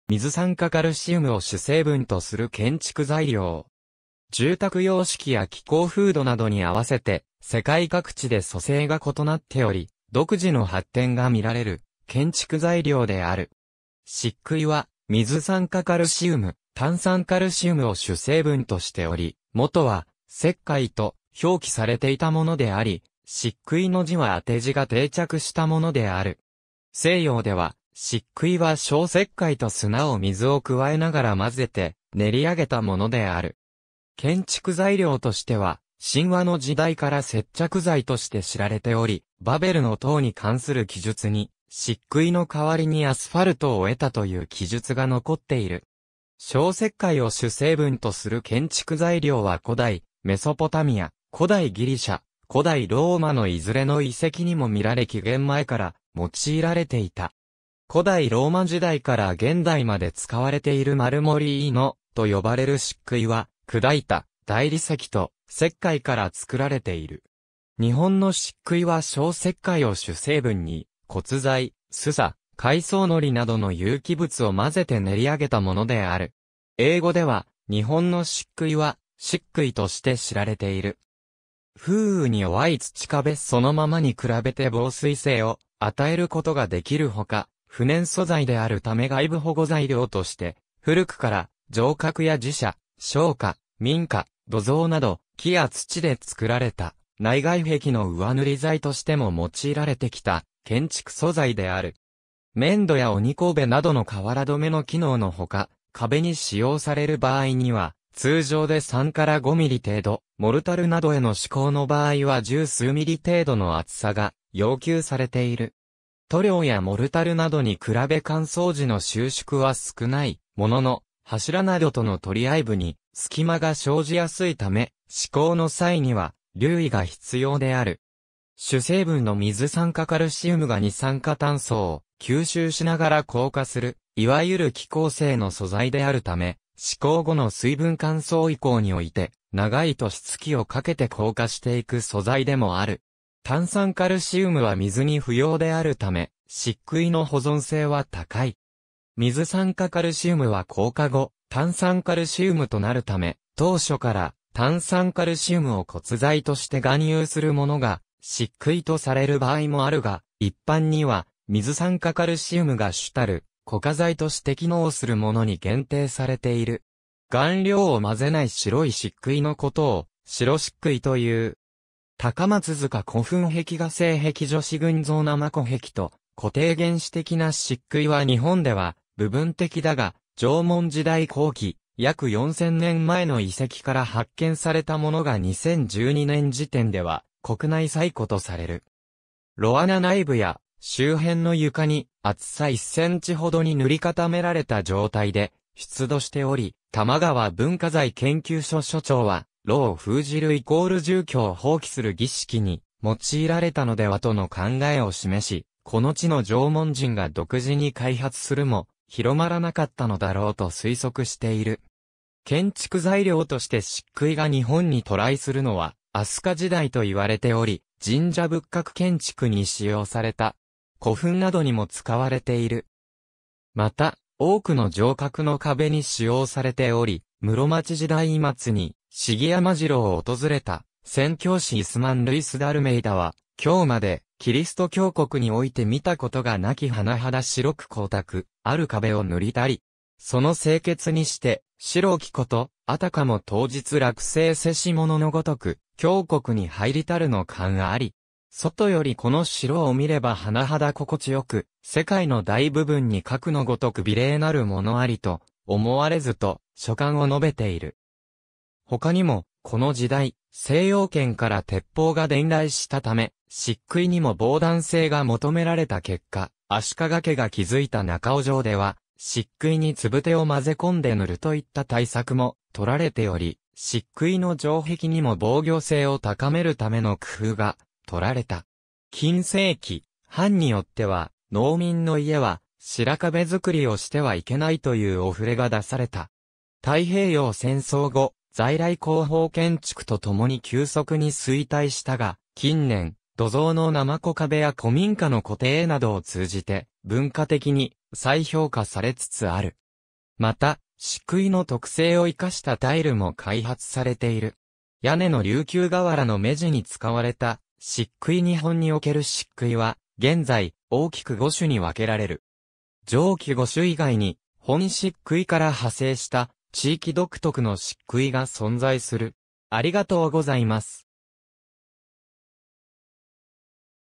漆喰
民家の壁に使われた漆喰 漆喰（石灰、しっくい）とは、水酸化カルシウム（消石灰）を主成分とする建築材料。
住宅様式や気候風土などに合わせて世界各地で組成が異なっており独自の発展がみられる建築材料である。
漆喰は、水酸化カルシウム・炭酸カルシウムを主成分としており、もとは「石灰」と表記されていたものであり、漆喰の字は当て字が定着したものである。
西洋では漆喰は消石灰と砂を水を加えながら混ぜて練り上げたものである。
建築材料としては、神話の時代から接着剤として知られており、バベルの塔に関する記述に「しっくいの代わりにアスファルトを得た」という記述が残っている。
消石灰を主成分とする建築材料は古代メソポタミア、古代ギリシャ、古代ローマのいずれの遺跡にもみられ紀元前から用いられていた。
古代ローマ時代から現代まで使われているマルモリーノと呼ばれる漆喰は、砕いた大理石と石灰から作られている。
日本の漆喰は消石灰を主成分に、骨材、すさ（麻）、海藻のりなどの有機物を混ぜて練り上げたものである。
英語では日本の漆喰はShikkuiとして知られている。
（en:Shikkui） 風雨に弱い土壁そのままに比べて防水性を与えることが出来るほか、不燃素材であるため外部保護材料として、古くから城郭や寺社、商家、民家、土蔵など、木や土で造られた内外壁の上塗り材としても用いられてきた建築素材である。
面土や鬼首などの瓦止めの機能のほか、壁に使用される場合には、通常で3 - 5ミリ程度、モルタルなどへの施工の場合は10数ミリ程度の厚さが要求されている。
塗料やモルタルなどに比べ乾燥時の収縮は少ないものの、柱などとの取り合い部に隙間が生じやすいため、施工の際には留意が必要である。
主成分の水酸化カルシウムが二酸化炭素を吸収しながら硬化(炭酸化)する、いわゆる気硬性の素材であるため、施工後の水分乾燥以降において長い年月をかけて硬化していく素材でもある。
炭酸カルシウムは水に不溶であるため、漆喰の保存性は高い。
水酸化カルシウムは硬化後、炭酸カルシウムとなるため、当初から炭酸カルシウムを骨材として含有するものが漆喰とされる場合もあるが、一般には水酸化カルシウムが主たる固化材として機能するものに限定されている。
顔料を混ぜない（で用いる）白い漆喰のことを、「白漆喰」という。
高松塚古墳壁画 西壁女子群像 なまこ壁とこて絵（新潟県長岡市の機那サフラン酒製造本舗土蔵） 原始的な漆喰（ほぼ石灰）は日本では、部分的だが、縄文時代後期、約4000年前の遺跡（千葉市、大膳野南貝塚）から発見されたものが2012年時点では国内最古とされる。
炉穴内部や周辺の床に厚さ1センチほどに塗り固められた状態で出土しており、玉川文化財研究所所長は、炉を封じる=住居を放棄する儀式に用いられたのではとの考えを示し、この地の縄文人が独自に開発するも広まらなかったのだろうと推測している。
建築材料として漆喰が日本に渡来するのは飛鳥時代といわれており神社仏閣建築に使用された。
古墳（高松塚古墳壁画等）などにも使われている。
また、多くの城郭の壁に使用されており、室町時代末（1565年）に信貴山城（奈良）を訪れた宣教師イスマン・ルイス・ダルメイダは、「今日までキリスト教国において見たことがなき甚だ白く光沢ある壁を塗りたり。其の清潔にして白きこと、あたかも当日落成せしものの如く、教国に入りたるの感あり。外より此城を見れば甚だ心地よく、世界の大部分にかくの如く美麗なるものありと思はれず」と、所感を述べている。
他にも、この時代、西洋圏から鉄砲が伝来したため、漆喰にも防弾性が求められた結果、足利家が築いた中尾城では、漆喰に礫（こいし）を混ぜ込んで塗るといった対策も取られており、漆喰の城壁にも防御性を高めるための工夫がとられた。
近世期、藩によっては、農民の家は白壁作りをしてはいけないというお触れが出された（『群馬県の歴史』 群馬県歴史研究会編 1970年 p.103）。
太平洋戦争後、在来工法建築とともに急速に衰退したが、近年、土蔵のなまこ壁や古民家のこて絵などを通じて文化的に再評価されつつある。
また、漆喰の特性を生かしたタイル（漆喰タイル）も開発されている。
屋根の琉球瓦の目地に使われた漆喰 日本における漆喰は現在、大きく5種に分けられる。
上記5種以外に、本漆喰から派生した地域独特の漆喰が存在する（肥後漆喰など）。